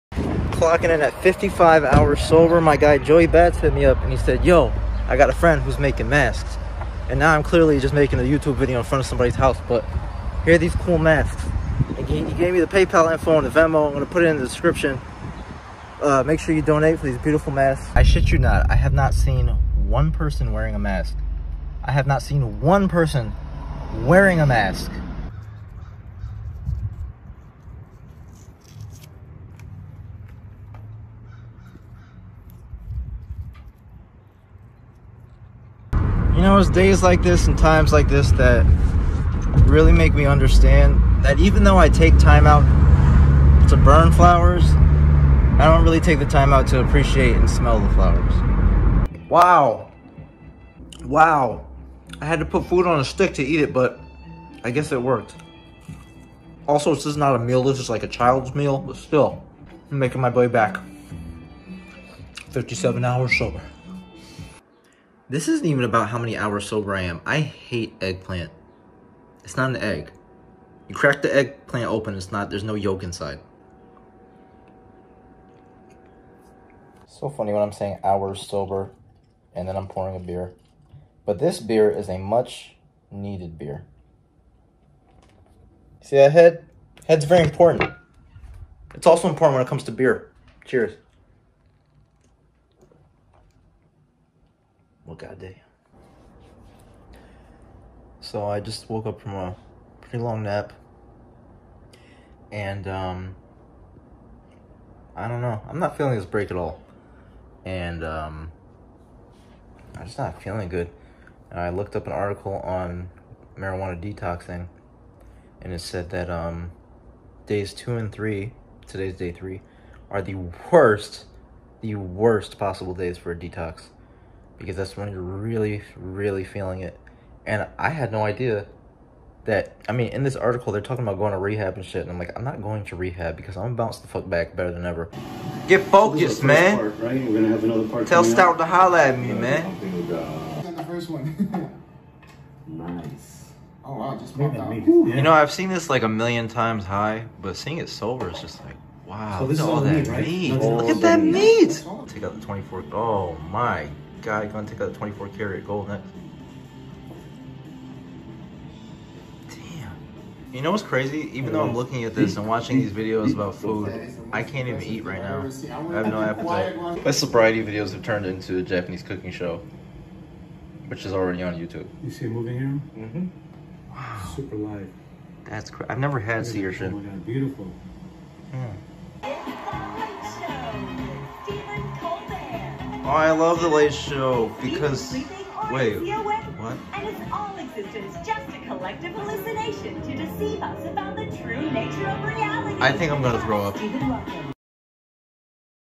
Quarantined Sobriety: Day 3 | Dabster Morgan
Dabster trudges through another day of sobriety and is really starting to feel it. Not all sunshine and flowers but don't count him out yet.

free homemade masks: @eat.your.beats tell her Dabster sent you and please donate!
snap/IG: @dabstermorgan